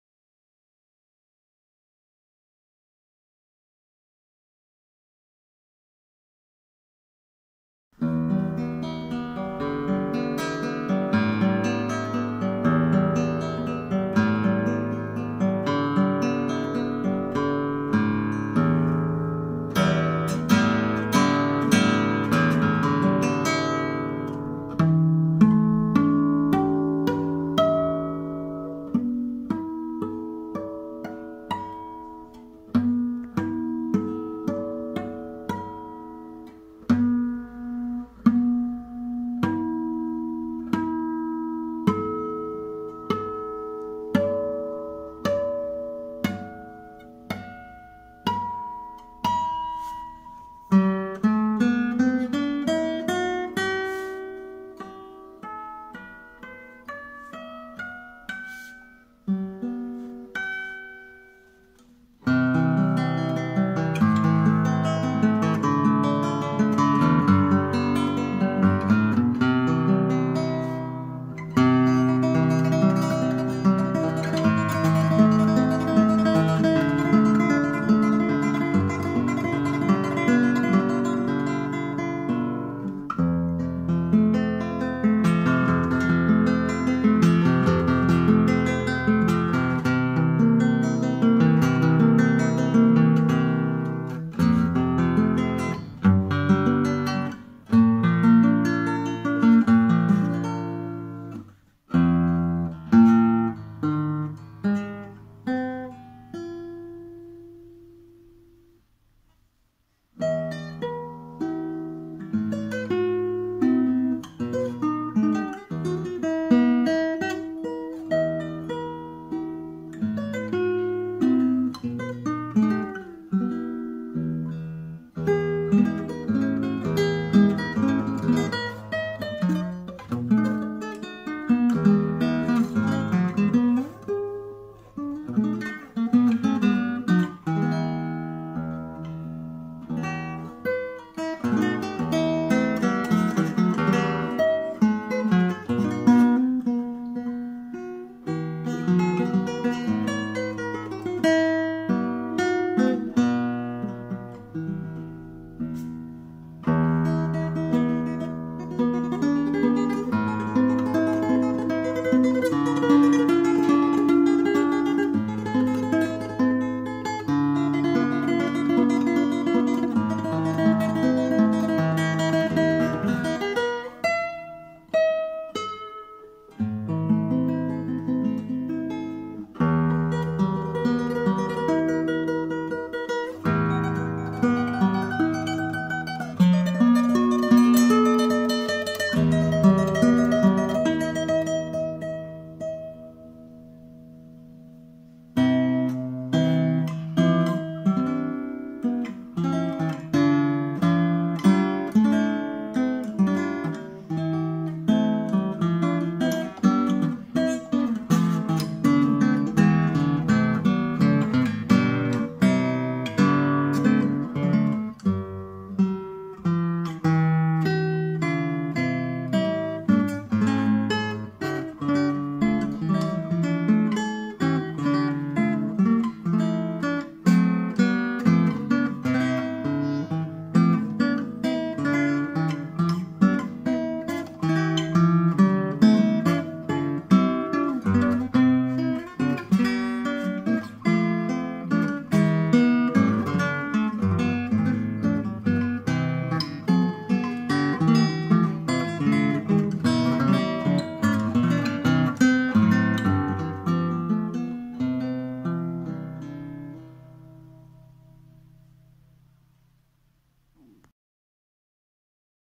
PRODIPE CLASSIC STUDENT CEDRO - prova con Giacomo Parimbelli
Il Maestro Giacomo Parimbelli presso il nostro negozio di strumenti musicali in provincia di Bergamo: Guitar Gear Dalmine (BG), prova una delle chitarre disponibili: La Prodipe Student 4/4 con tavola in cedro massello, fasce e fondo in mogano, versione non amplificata, chitarra ideale sia per il principiante o per l'amatore, dotata di un suono caldo e potente, si presta per poter suonare qualsiasi brano rimanendo intonata e facile da suonare in ogni punto della tastiera.
Il maestro ci mostra come le note e gli armonici siano perfettamente suonabili in ogni parte del manico, e successivamente esegue musiche di Tarrega, Pezzoli, Mozzani e Fernando Sor